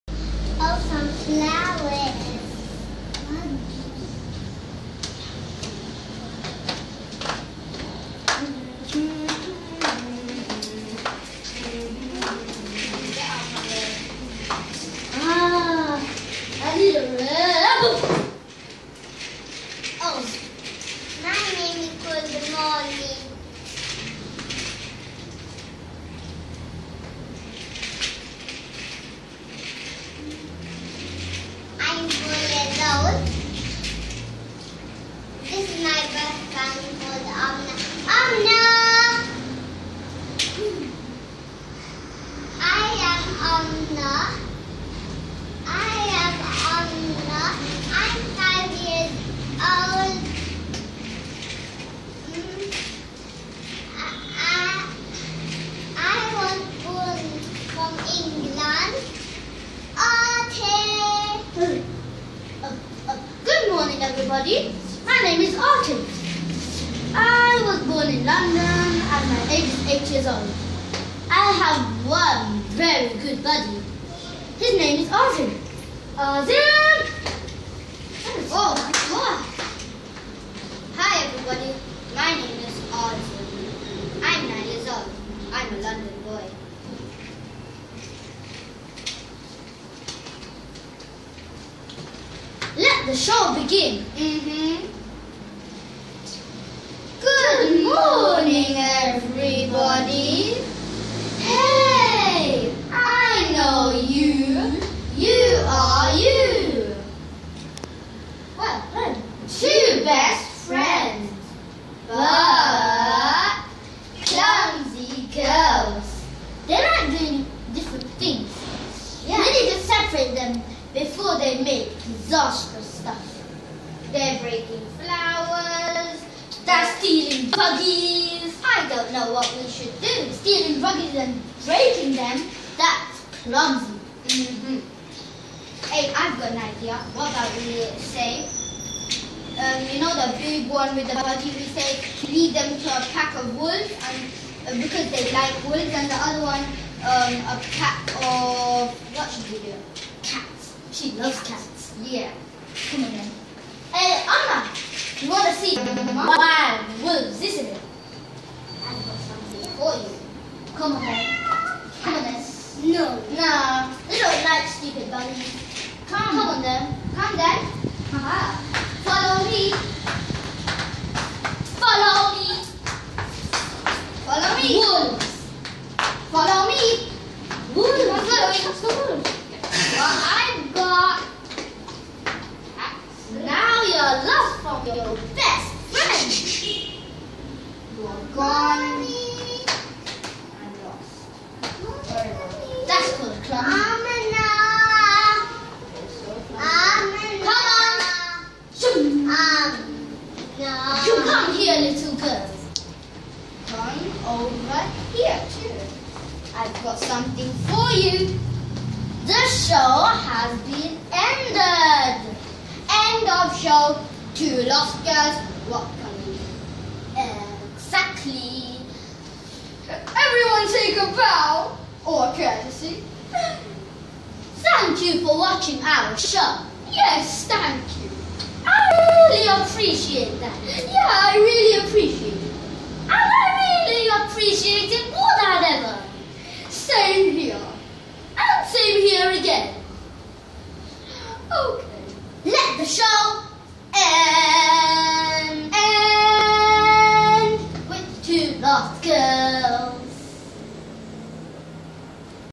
London Sibling Show
The siblings have fun in their grandparents house in Kandy.